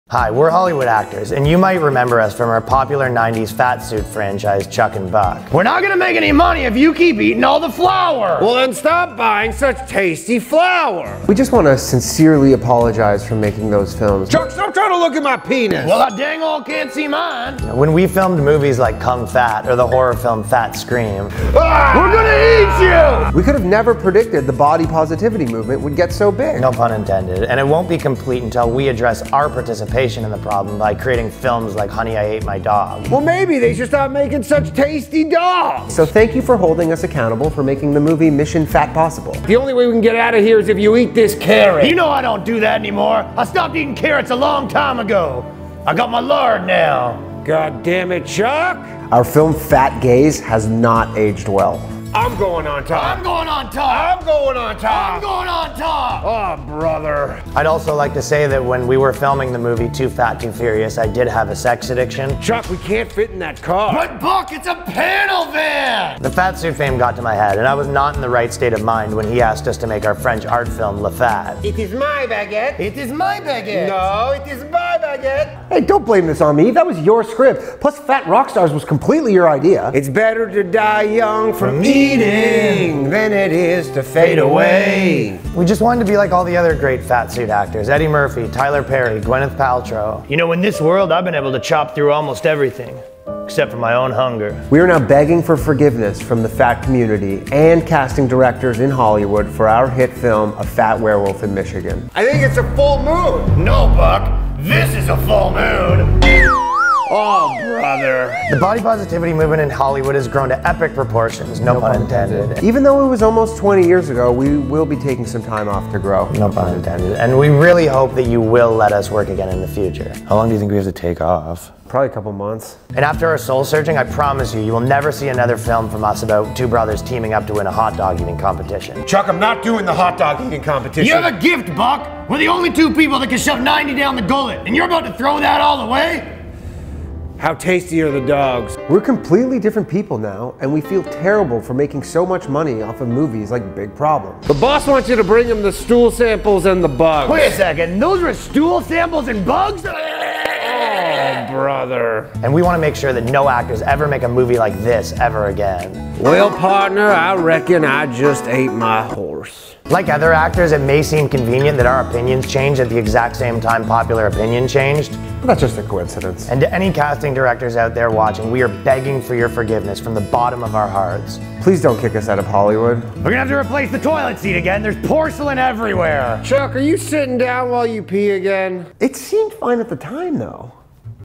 Hollywood Actors Apologize For Fat Suit Movies
These actors are appalled about their participation in fat suit movies.